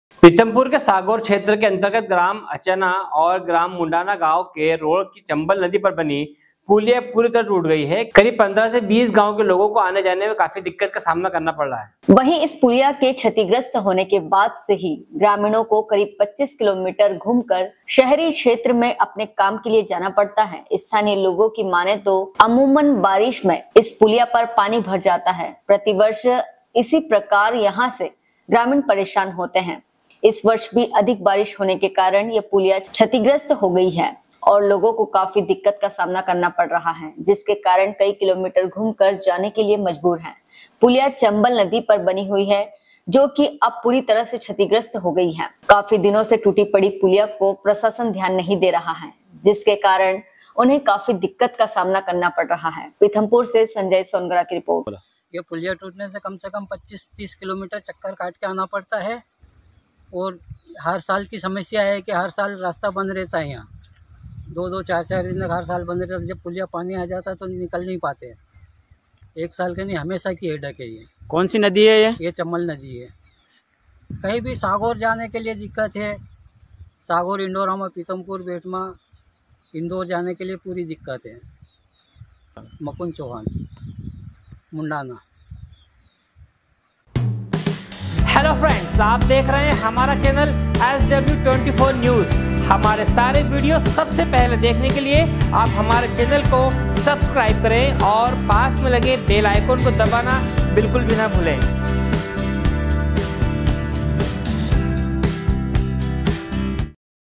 पीथमपुर, चंबल नदी पर पुलिया टूटने से 15 गांवों के लोगों को हो रही मुश्किल
पीथमपुर के सागौर क्षेत्र में चंबल नदी पर बनी पुलिया पूरी तरह से टूट गई है, जिससे ग्राम अचाना और मुंडाना सहित 15 से 20 गांवों के लोगों को गंभीर समस्याओं का सामना करना पड़ रहा है। अब ग्रामीणों को 25 किलोमीटर सफर करना पड़ रहा है। बारिश के कारण यह पुलिया अक्सर जलमग्न हो जाती है, और इस बार की अधिक बारिश ने इसे क्षतिग्रस्त कर दिया। प्रशासन की अनदेखी से स्थानीय लोग परेशान हैं। पीथमपुर से संजय सोनगरा की रिपोर्ट।